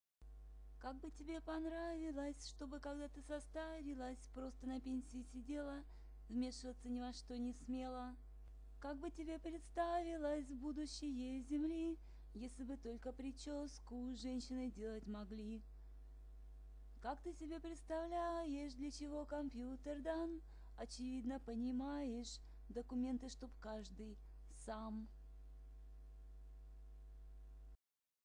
женские профессии?
Как бы тебе понравилось
Чтобы когда ты состарилась
Просто на пенсии сидела,
Вмешиваться ни во что не смела?

Как бы тебе представилось
Будущее Земли
Если бы только причёску
Женщины делать могли?

Как ты себе представляешь
Для чего компьютер дан,
Очевидно, понимаешь,
Документы чтоб каждый - САМ!